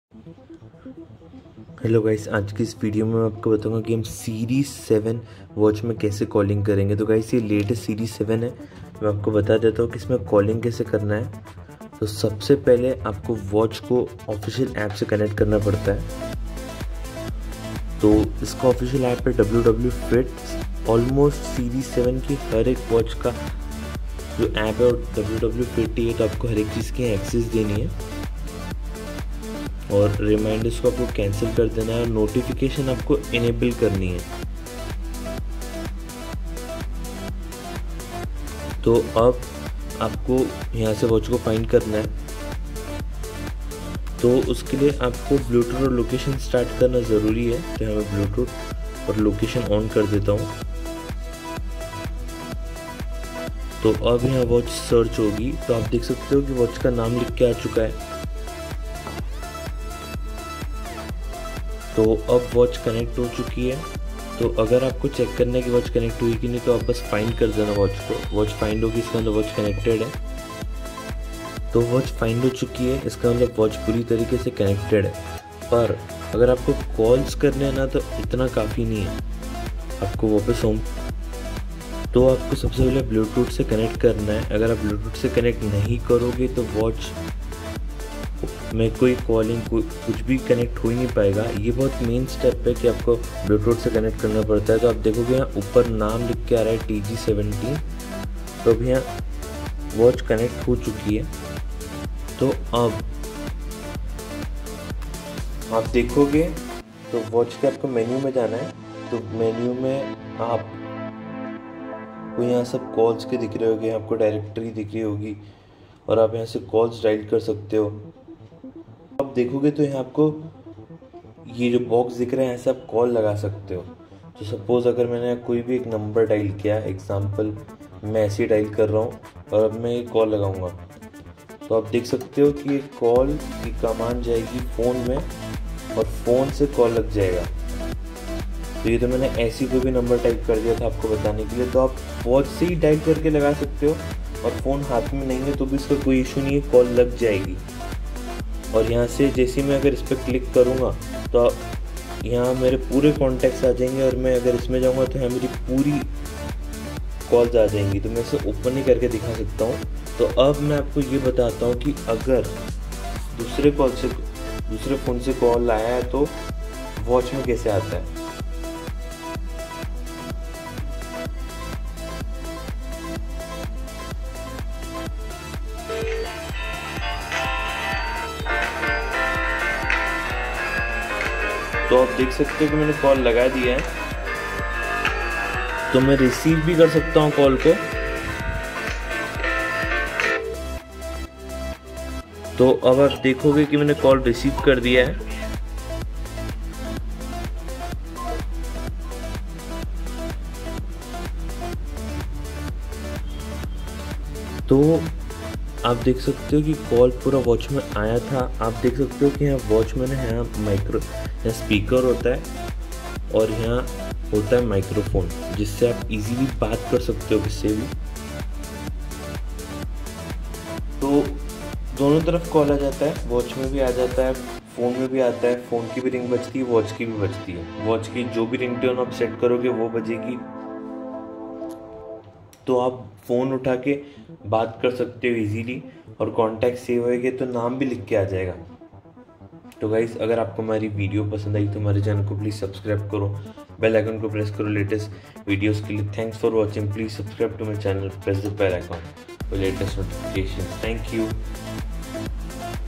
How to make call from series 7 clone | How to make call from apple watch Iwatch series 7 clone
This Video Is about series 7 calling